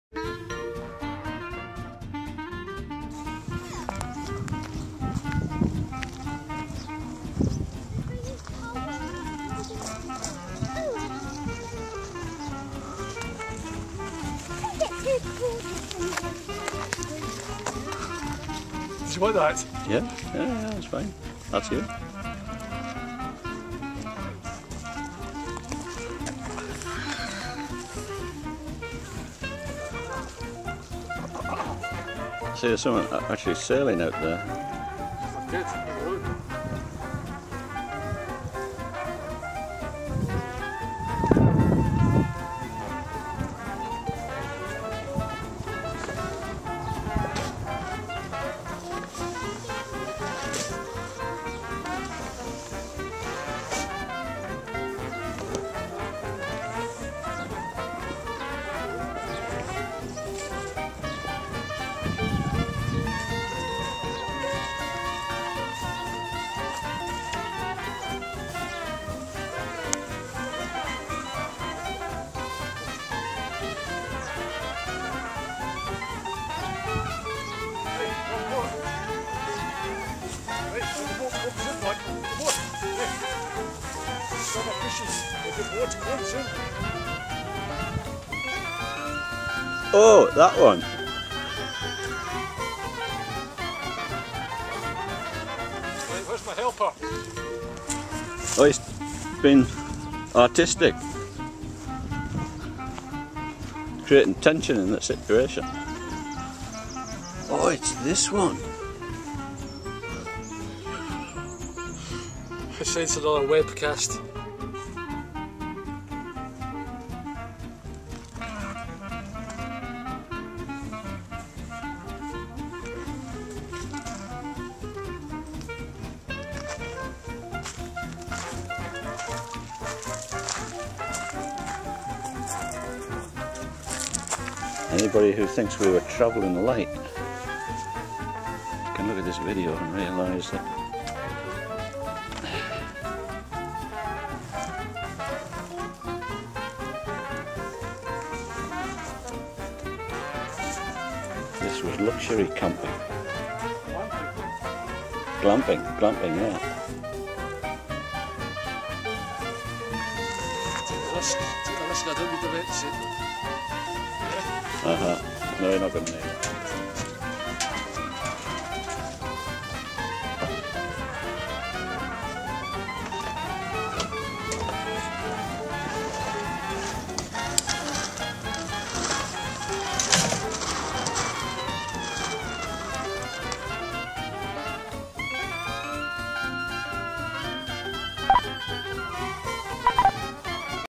2 ard men on a loch
john goes dingy sailing for the first time; rob has done it before (many times)